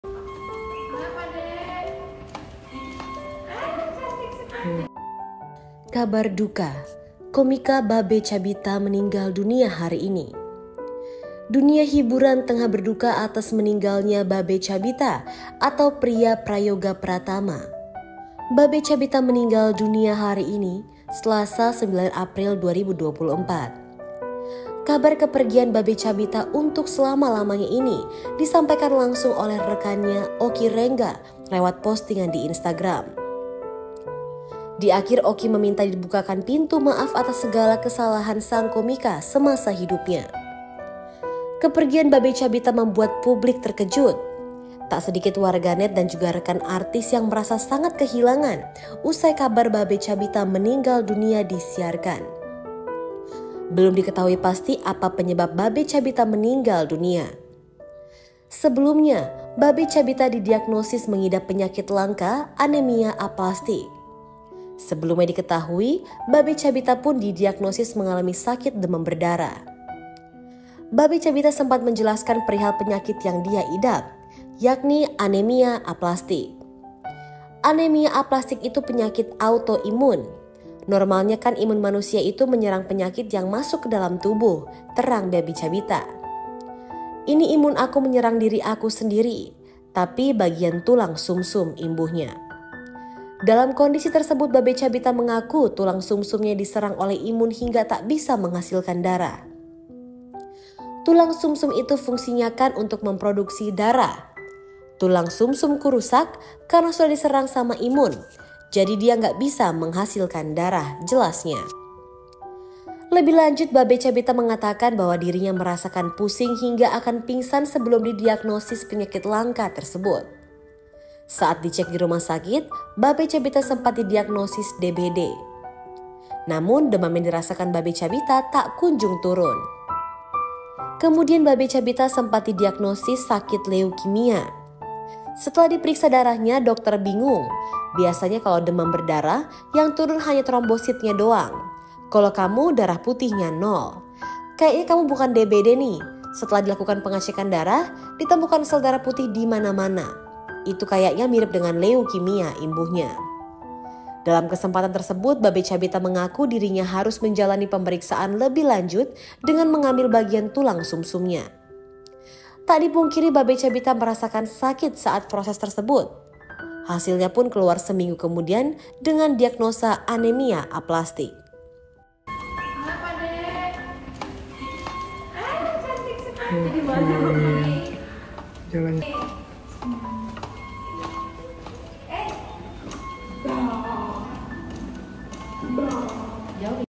Detik - Detik Babe Cabita Meninggal Karena Penyakit Langka
Dunia hiburan tengah berduka atas kabar Babe Cabita atau Priya Prayogha Pratama meninggal dunia.

Komika Babe Cabita meninggal dunia hari ini Selasa, 9 April 2024.

Kabar kepergian Babe Cabita untuk selama-lamanya ini disampaikan langsung oleh rekannya, Oki Rengga lewat postingan di Instagram.

(nad)